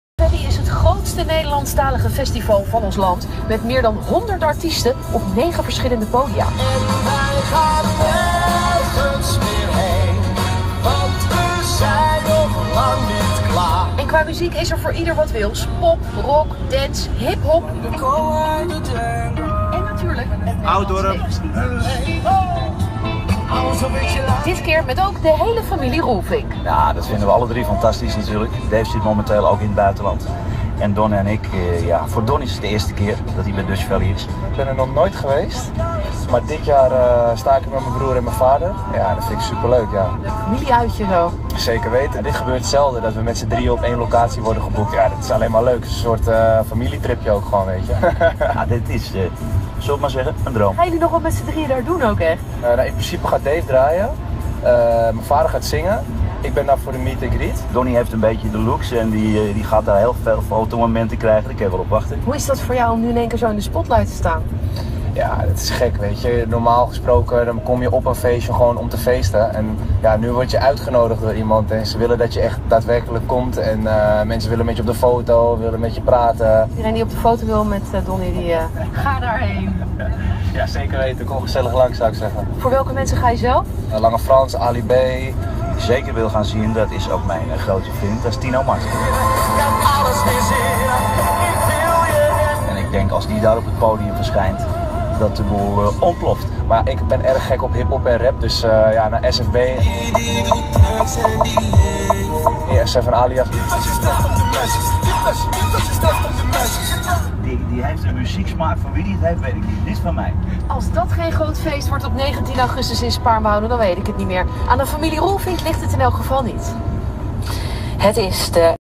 De 3D's op Dutch Valley 2017!
Meet de Roelvinkjes! Op de Bestorming stage, hosted door Cafe La Bastille staan ze alle drie: Dries zingt, Dave draait en Donny doet de meet & greet.

Dutch Valley Festival
Locatie: Velsen Valley Spaarnwoude

Dit is het officiële YouTube-kanaal van Dutch Valley Festival.